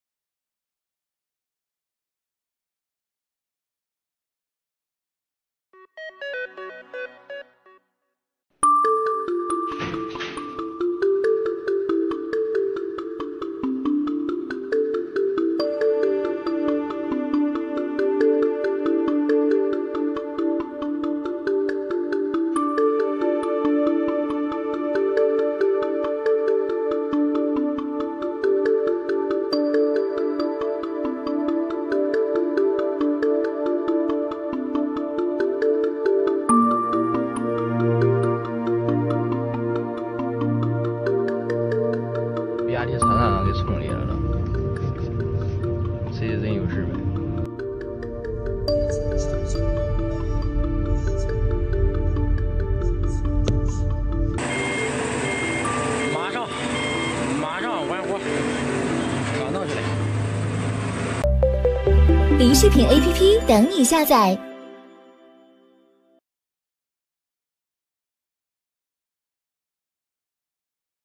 Truck crashes into a control room
Happened at this Location: Zaozhuang, Shandong, China
On April 28, in a large truck crashed into the east platform of the BRT station in the hot spring town. Then, the staff ran out of the control room. The guardrail on the side of the platform was hit and the scene was fierce. The staff introduced that the duty officer was sitting in the control room at that time, and the front of the car did not hit the direction, which was particularly thrilling.

BuckleupDaily & Drive Safely!
Latest Car Accidents and Crashes
Bucklupdaily.org
!قد بأمان!